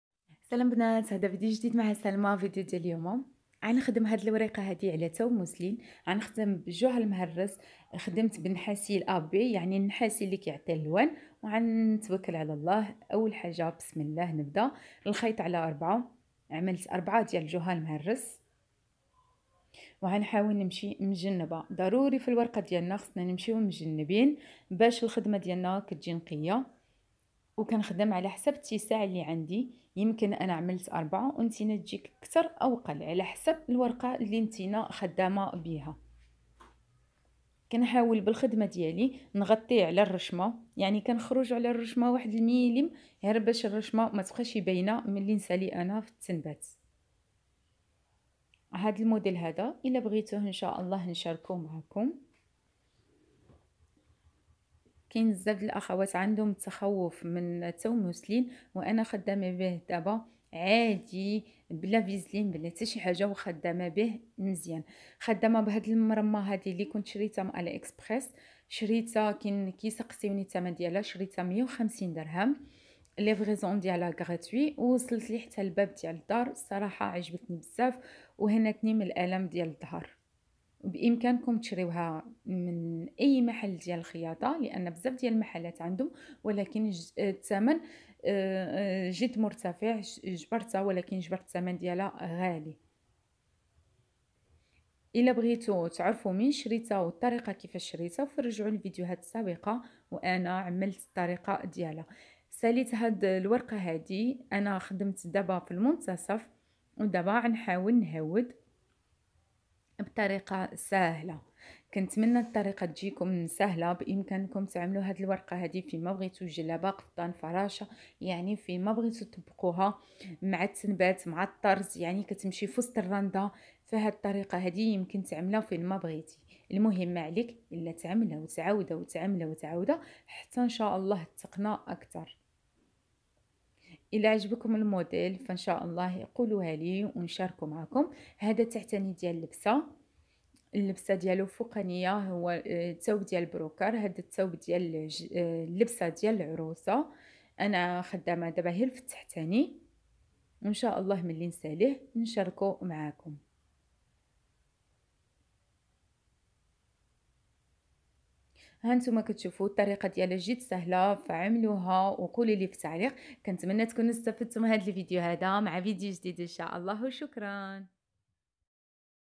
ta3lim tnbat l39i9 m3a salma salim
ta3lim tnbat l39i9 m3a salma salim f tawb moslin 9ftan mnbt bl39i9
طريقة سهلة لتنبات لعقيق في توب موسلين وهي طريقة سهلة لتعليم التنبات للمبتدئات في فن التنبات مع سلمى سالم